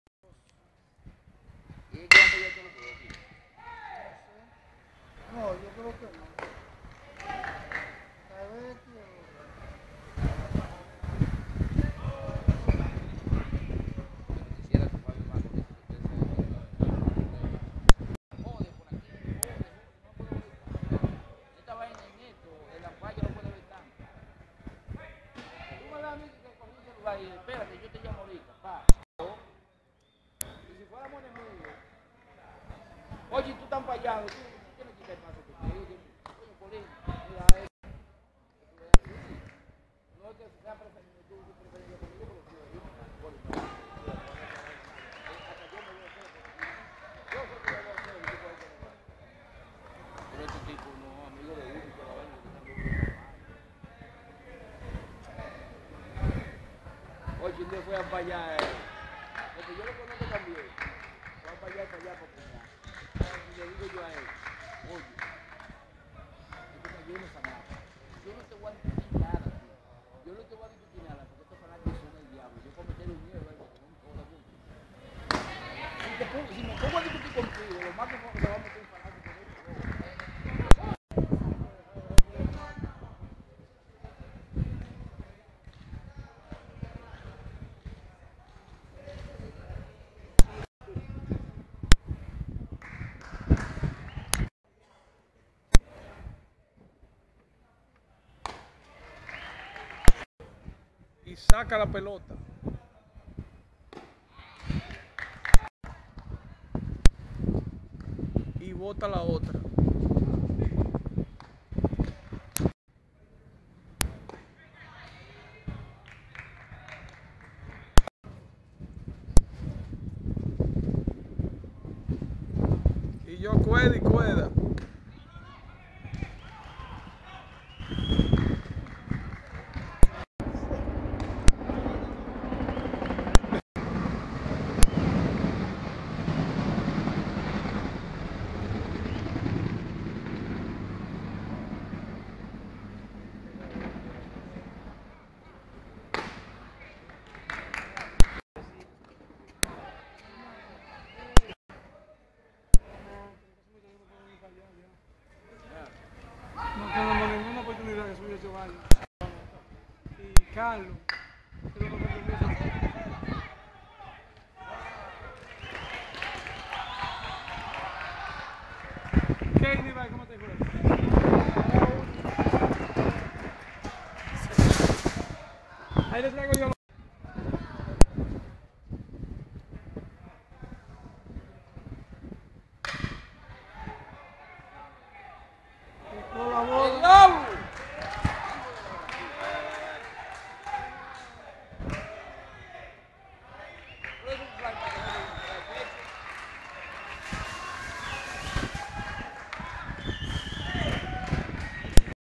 INTERBARRIAL.mp4
ACTIVIDAD DEL MIERCOLES 9/5/12 DEL BEISBOL INTERBARRIAL DE SANTIAGO ORGANIZADO POR LA ABOS